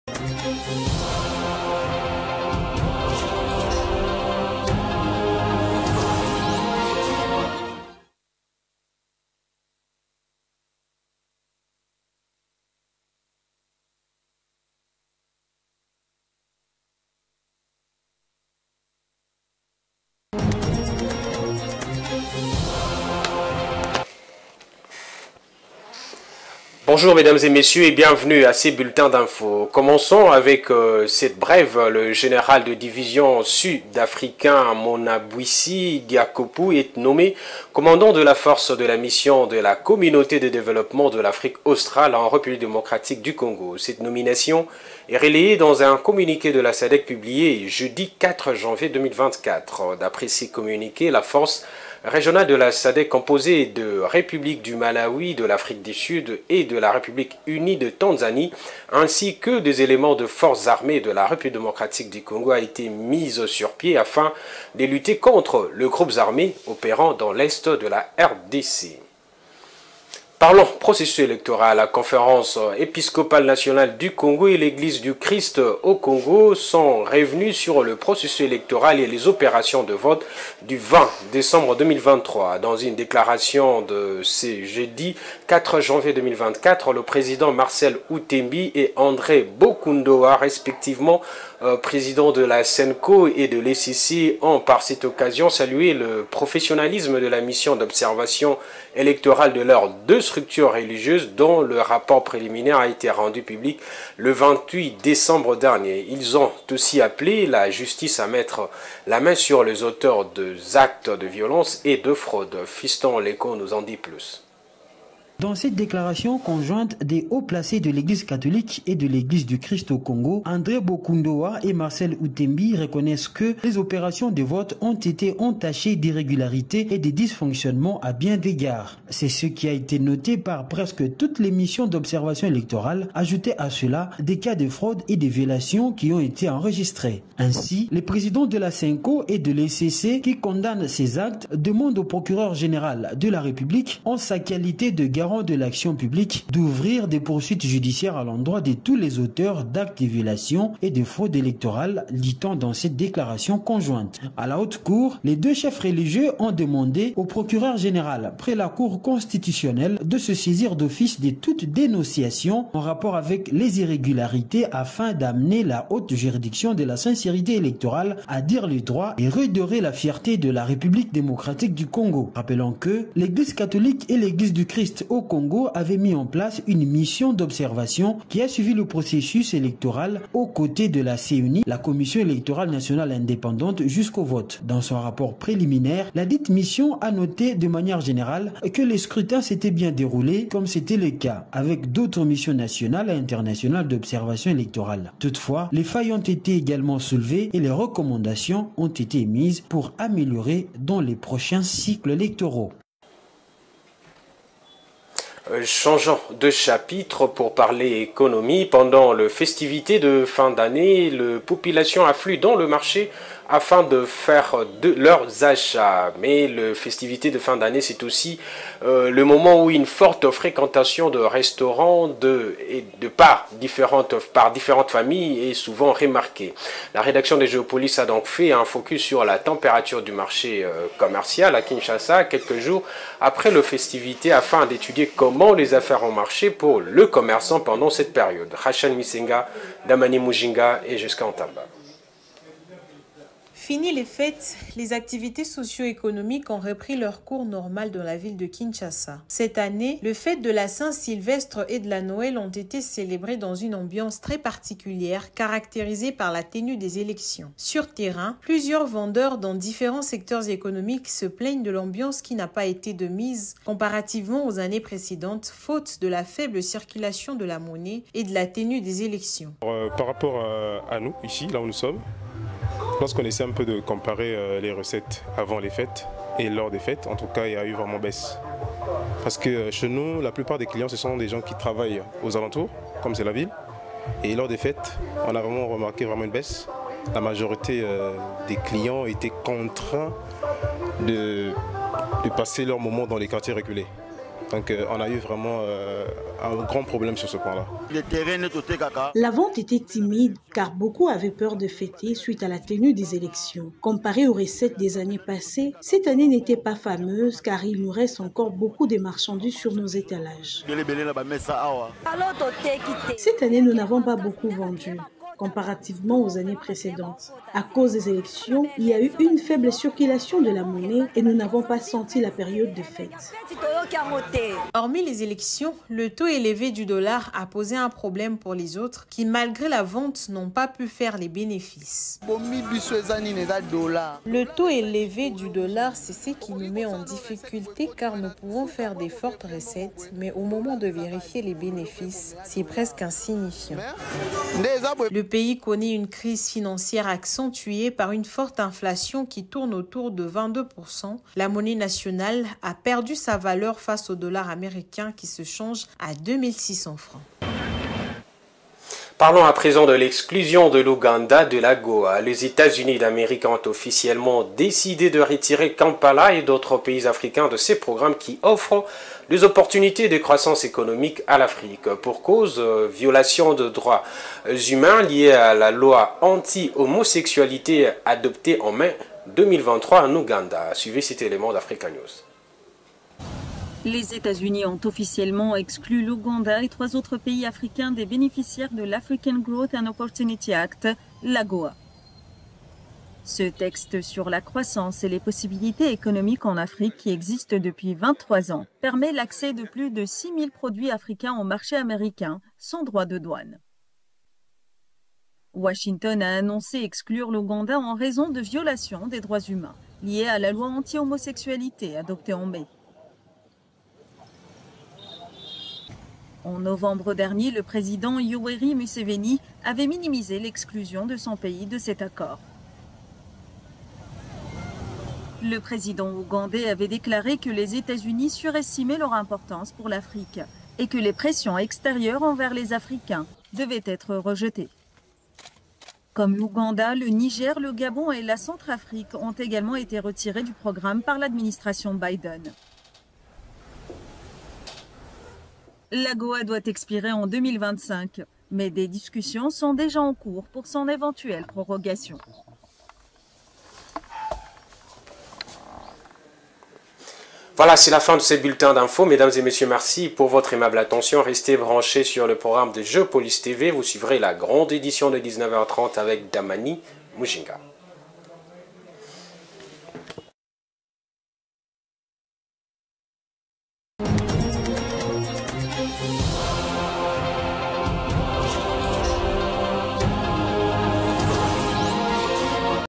BULLETIN D'INFOS : ELECTIONS2023|LaCENCOetL'ECCappellentLaJusticeàPoursuivreLesAuteursDesCasDeFraude
Edition du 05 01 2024 présentée par Mr. Djodjo Mulamba